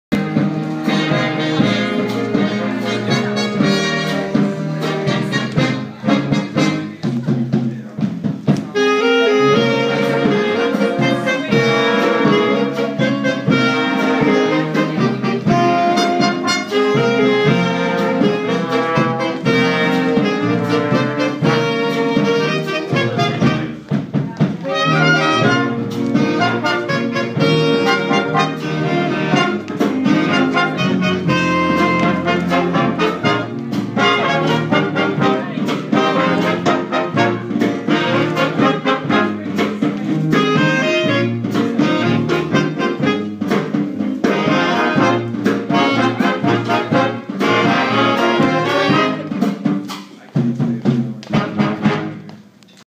Pine River Jazz Band
Performing at Leo's UpStairs Lounge.
The next Dance is May 10, 2014